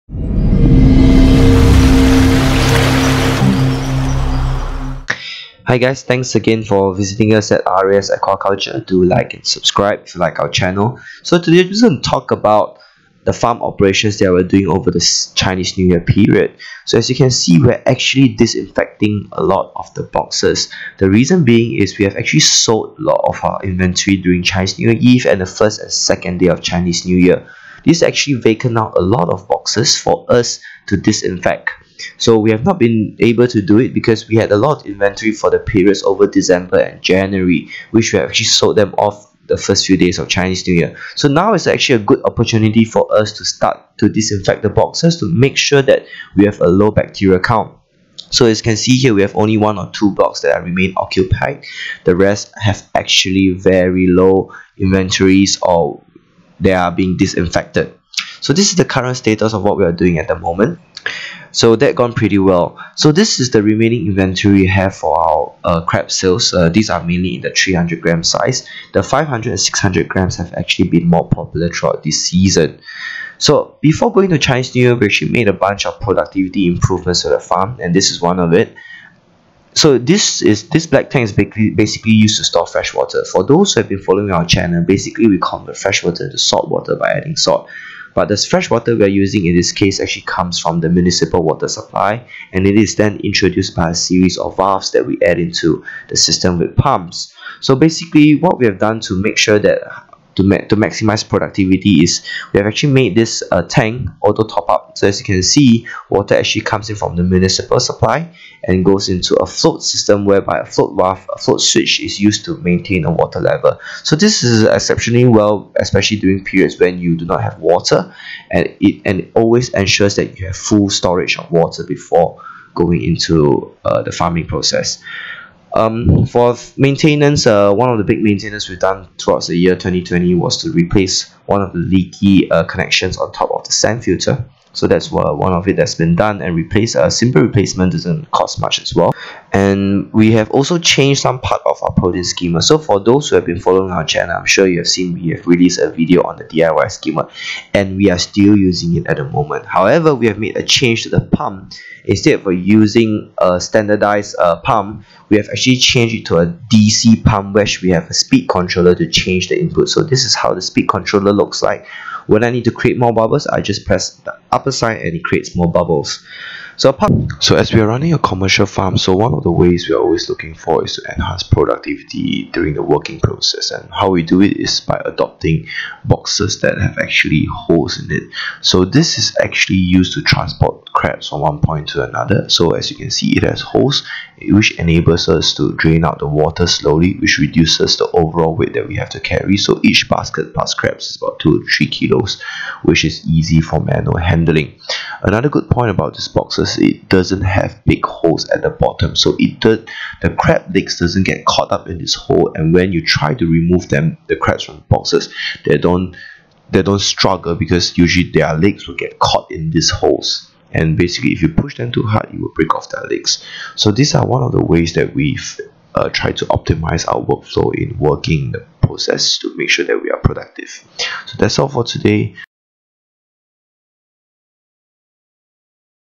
Mud Crab Vertical Farming Operations, Maintenance and Productivity Hacks
Hello Everyone!

Check out this video on the mud crab vertical farming operations, maintanence and productivity hacks for the year 2020!